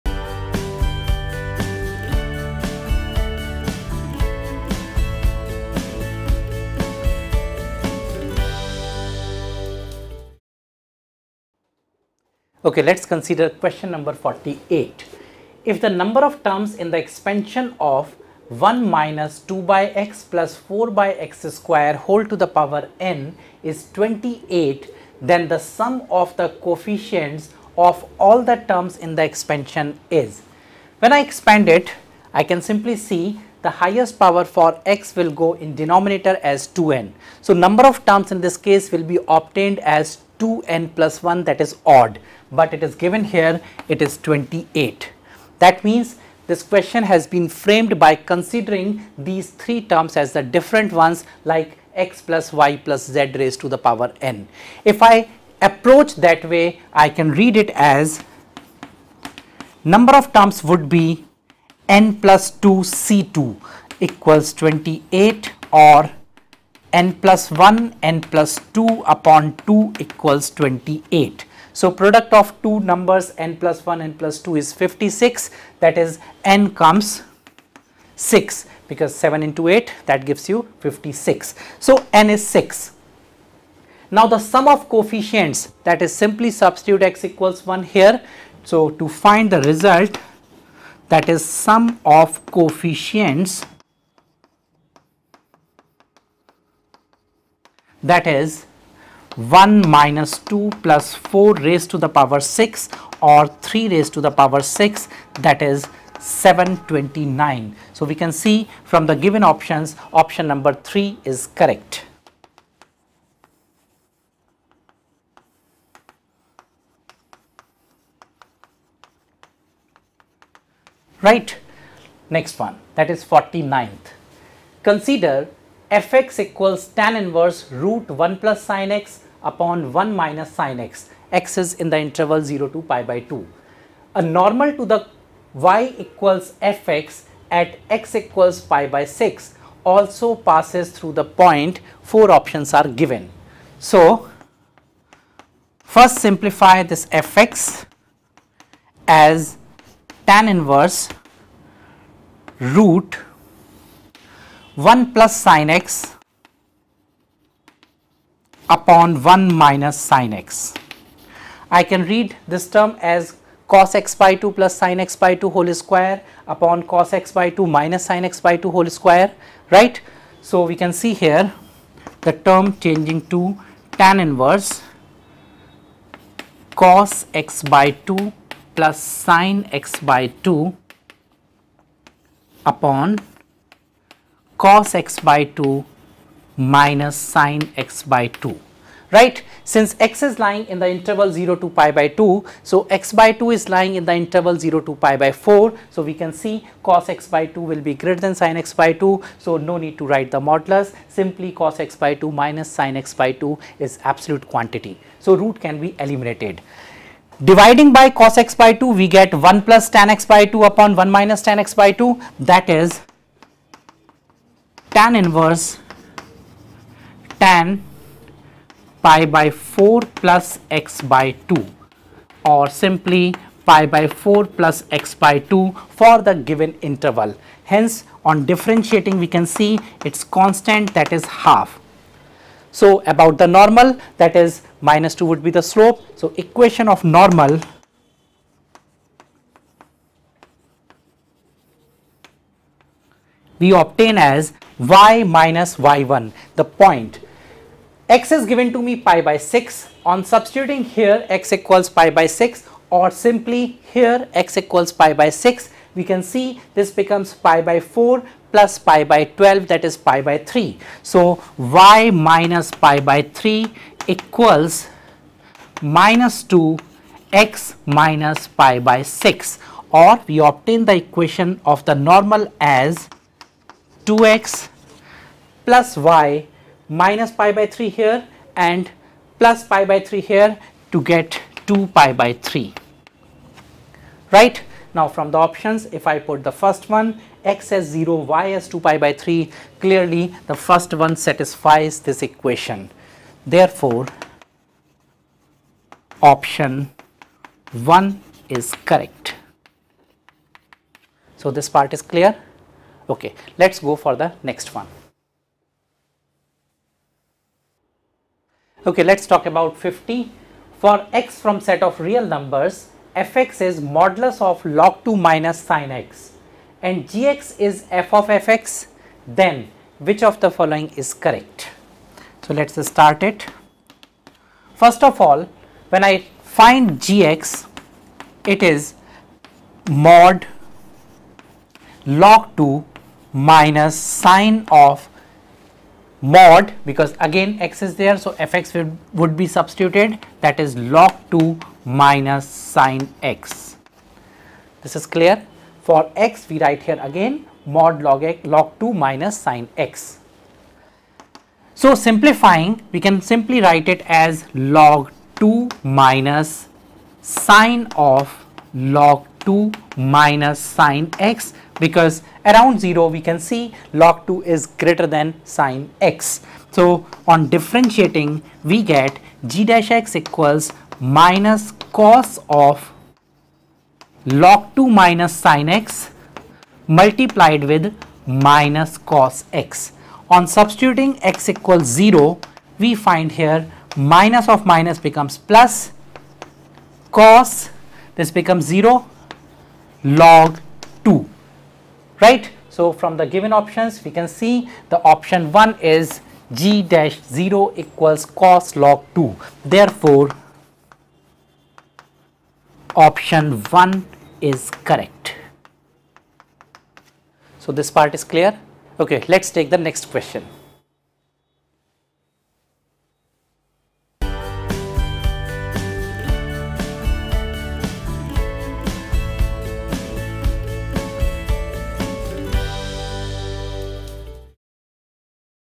JEE-Main 2016 Mathematics Solution [Q. 48-50] By Aakash Faculty
Find JEE (Main) 2016 Video Solutions of Mathematics Paper from Question No. 48-to-50 (Code-F) by Aakash. [Apply here for JEE (Advanced) Crash Course